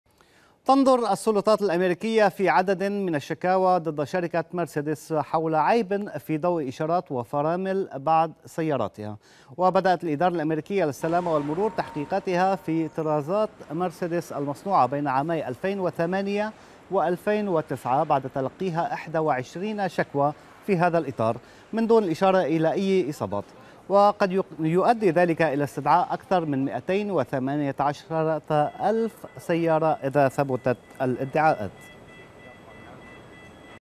أميركا.. شكاوى ضد سيارات مرسيدس
تنظر السلطات الأميركية في عدد من الشكاوى ضد شركة مرسيدس حول عيب في ضوء إشارات وفرامل بعض سياراتها. وبدأت الإدارة الأميركية للسلامة والمرور تحقيقاتها في طرازات مرسيدس المصنوعة بين عامي 2008 و2009.